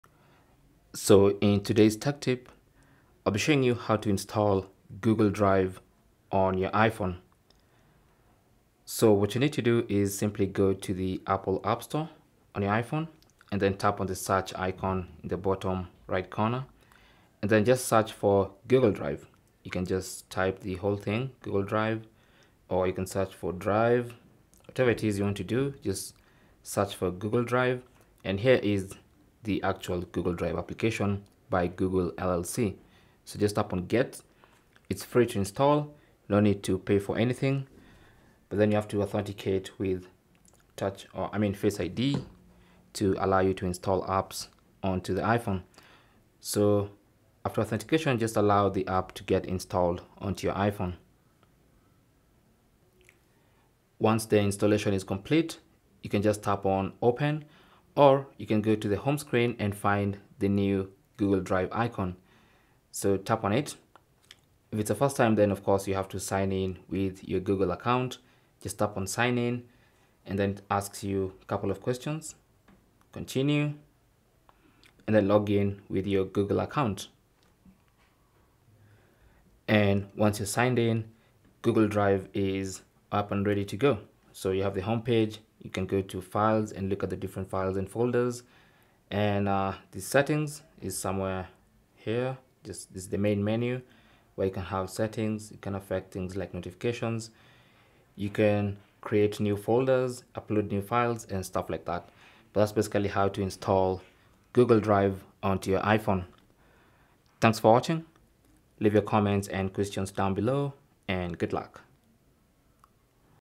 How To Install Google Drive On iPhone
Learn how to install Google Drive on iPhone.

Google Drive is an incredibly powerful and convenient cloud storage solution that allows you to store, access, and share your files across multiple devices seamlessly. Whether you want to back up important documents, share photos with friends and family, or collaborate on projects, Google Drive has got you covered.

Luckily, you can get Google Drive on your iPhone and enjoy the features that it has to offer. To install Google Drive on your iPhone, head to the App Store and tap on the search icon at the bottom right corner of the screen to search for Google Drive.

Type in Google Drive and from the search result, choose the one from Google. Tap on "Get" and when prompted, type in your Apple ID password. Once done, wait for the installation process to complete.

Once installed, you can launch the app by tapping on the "Open" button. Alternatively, you can find Google Drive on your home screen and then tap on it to launch it.

Galaxy S23 Ultra DUAL SIM 256GB 8GB Phantom Black:

Samsung Galaxy A14 (Factory Unlocked):

Cell Phone Tripod Adapter